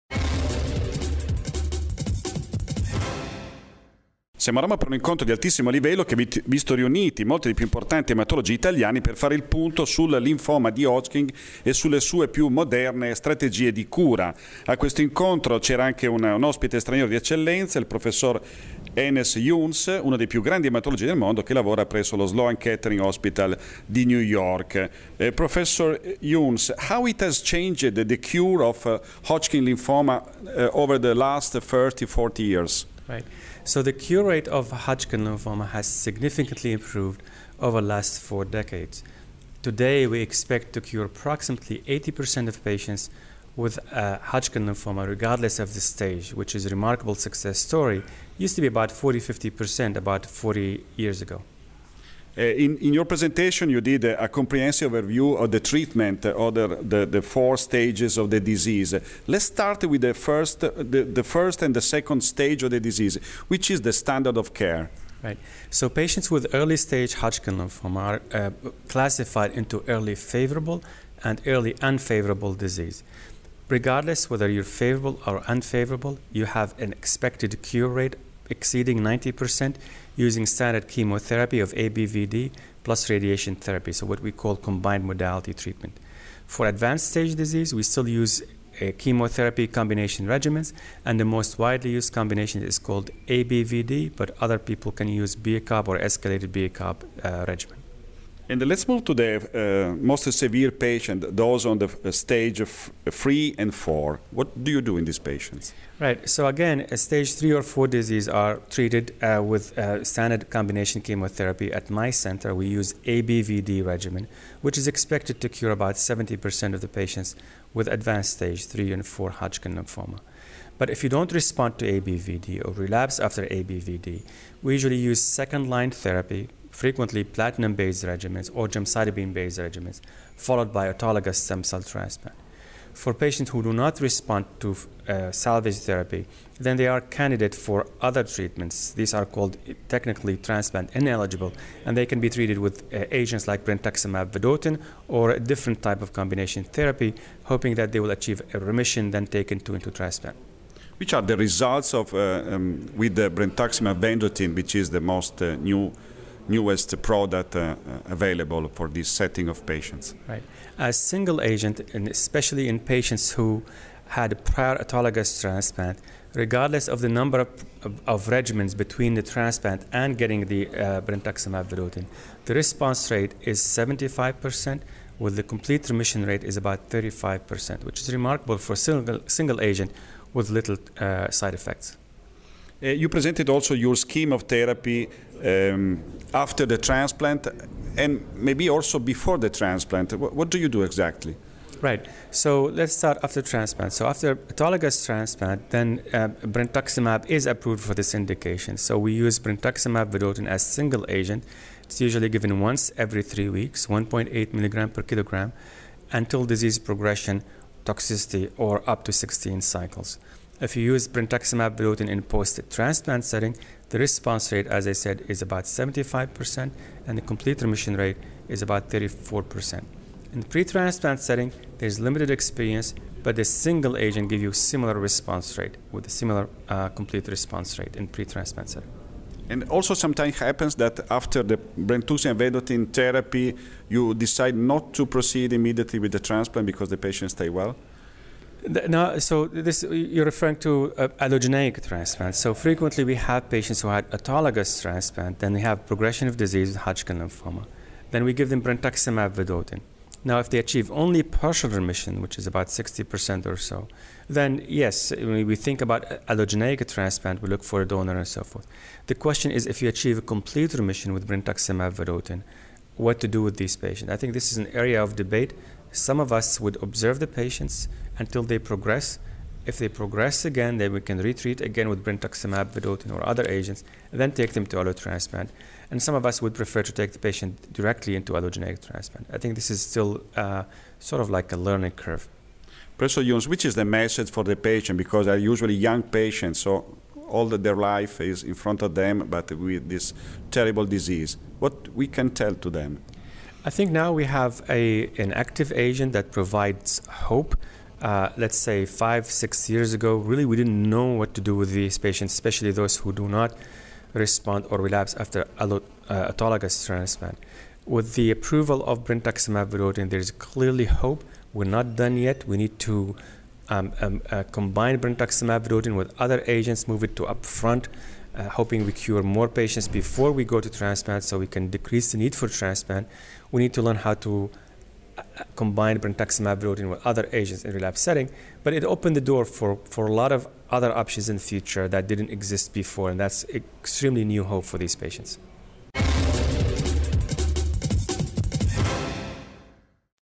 Come si cura il linfoma di Hodgkin: intervista a uno dei più grandi ematologi del mondo
Intervista al Prof. Anas Younes,
Chief of Memorial Sloan Kettering's Lymphoma Service in the Division of Hematologic Oncology
Department of Medicine, New York, NY